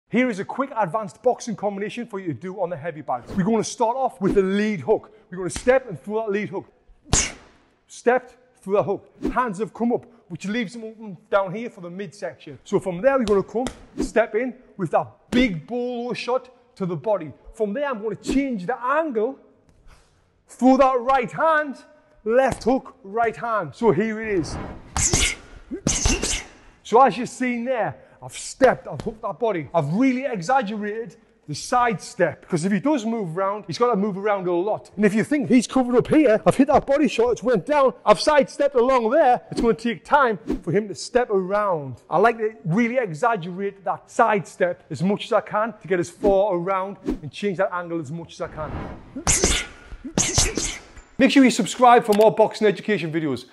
Advanced Boxing Combo You Can Learn in 10 Minutes
Tony Jeffries shares an Advanced Boxing combo you can practice on a heavy bag that can be learned in just 10 minutes. There are many boxing combinations you can practice whether you are a beginner or amateur boxer but this combination is an advanced realistic combination.